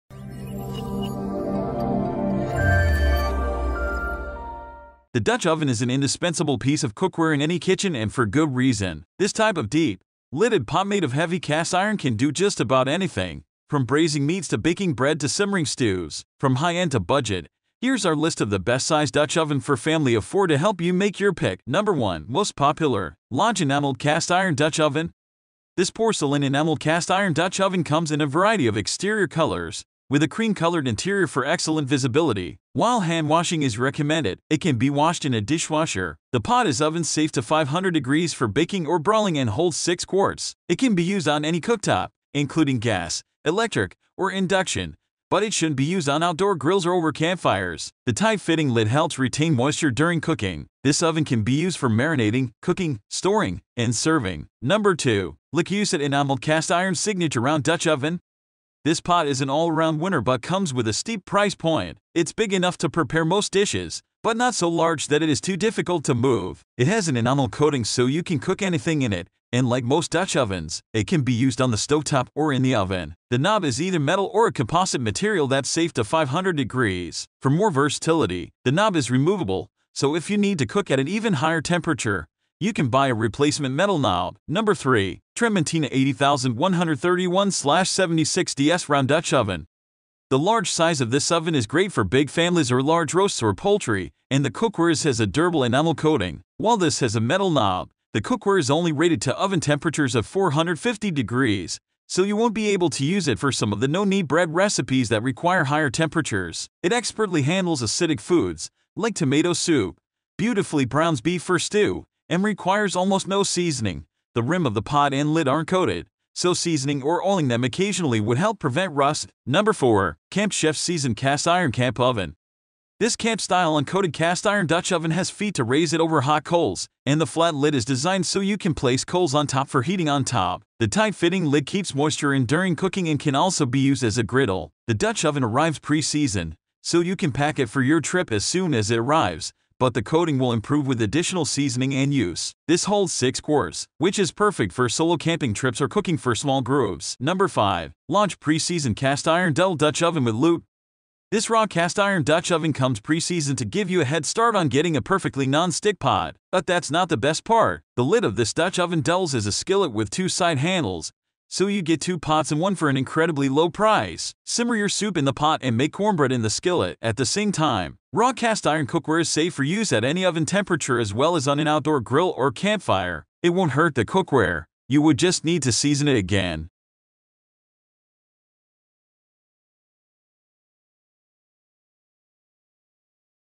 Top 5 Best Size Dutch Oven for Family of 4 Review in 2023
best size dutch oven for family of 4 featured in this Video:

0.23NO. 1. Lodge Enameled Cast Iron Dutch Oven
0.56NO. 2. Le Creuset Enameled Cast Iron Signature Round Dutch Oven
1.29NO. 3. Tramontina 80131/076DS Round Dutch Oven
2.00NO. 4. Camp Chef Seasoned Cast Iron Camp Oven
2.28NO. 5. Lodge Pre-Seasoned Cast Iron Double Dutch Oven With Loop

What is Dutch oven for family of 4?

Dutch oven for family of 4 is a type of oven that can be used for four family members. So for a family of four, you would want something that is a minimum of 4 quarts. And then with rounding up, the ideal size would be somewhere in the 5-6 quart capacity. But an important thing to consider is whether you also often cook for extended family or guests.

What is the Best  Dutch oven for family of 4 to buy?

If you are looking for the top  Dutch oven for family of 4, then you are in the right place.